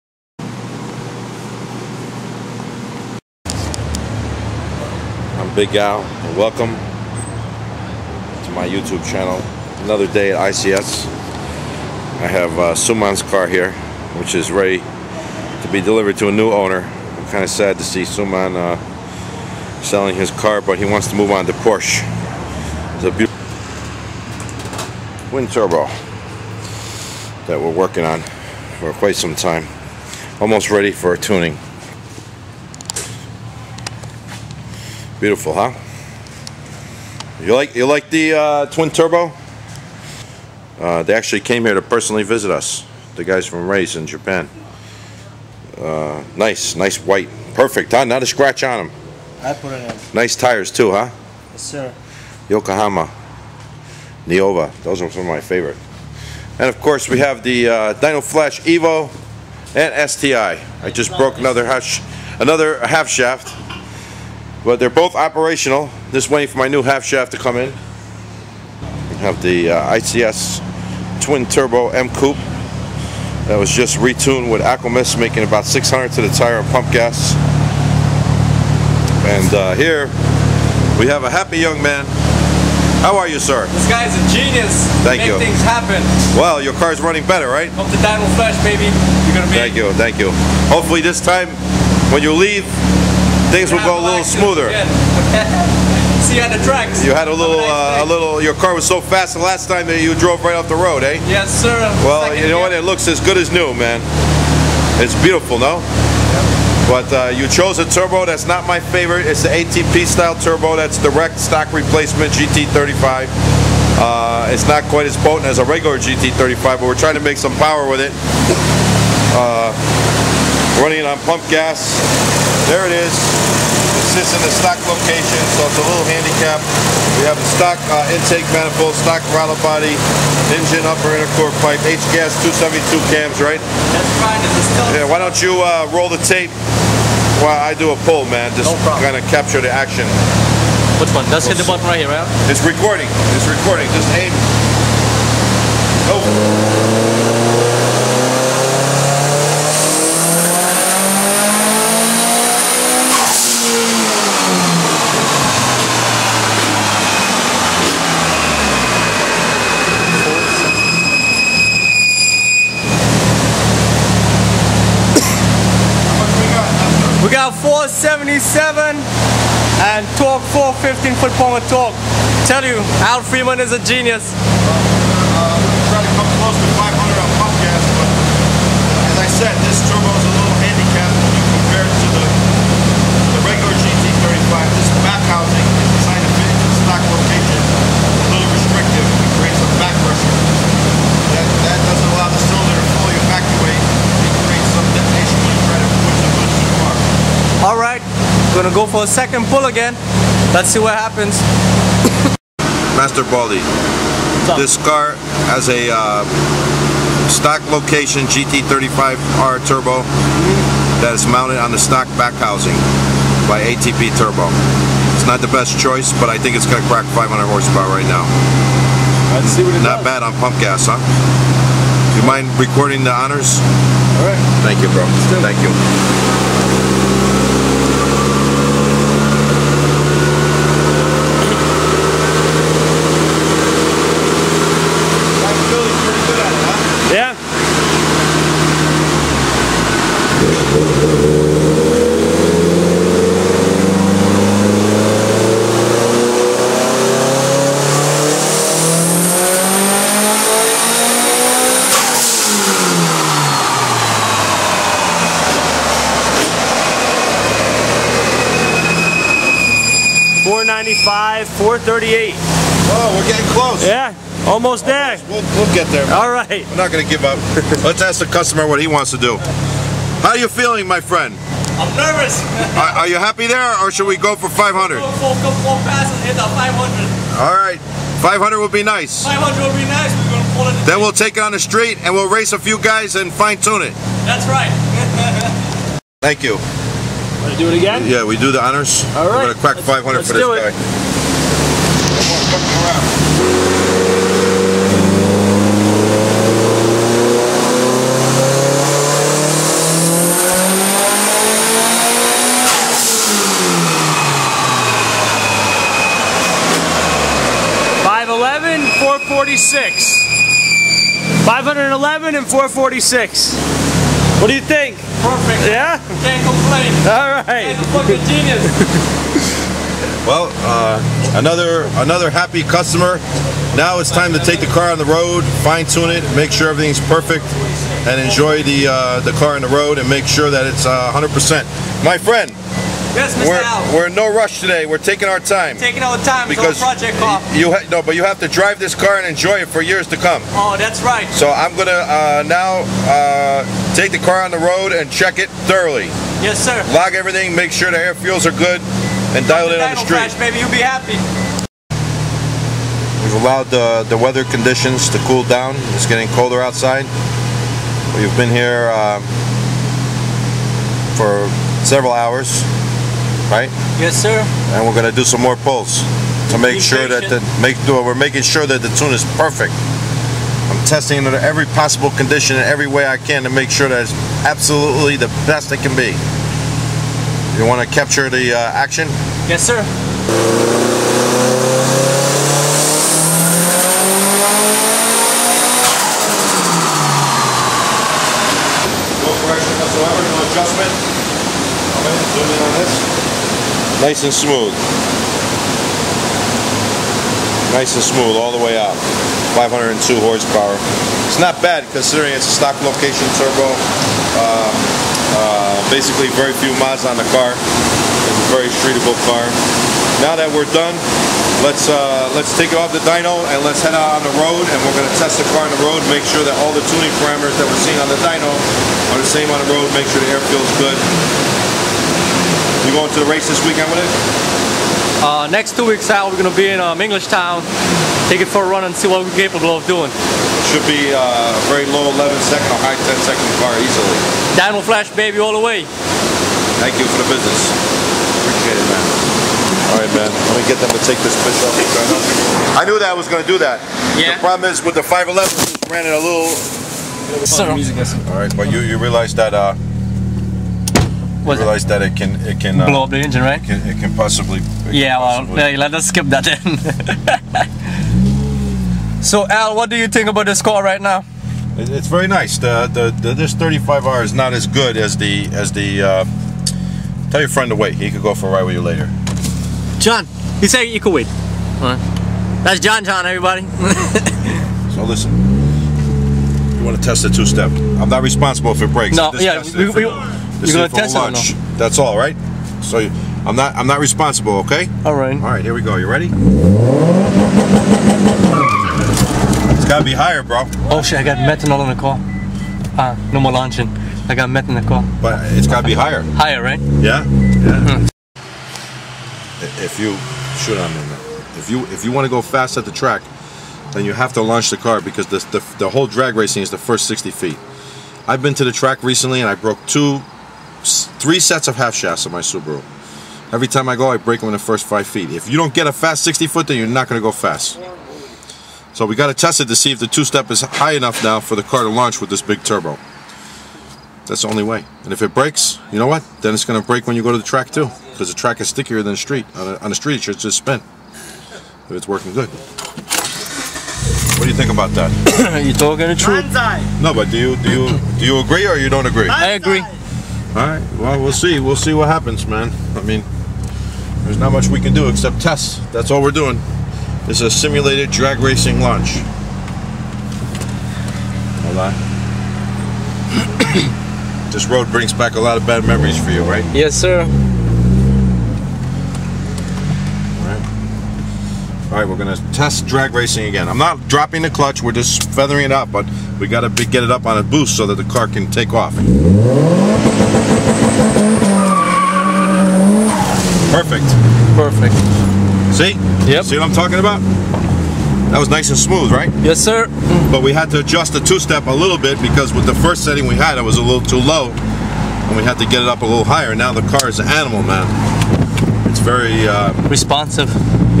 Tuning a Evo 8 with ATP Gt35
We push the ATP stock replacement Gt35 turbo to 500 whp on pump gas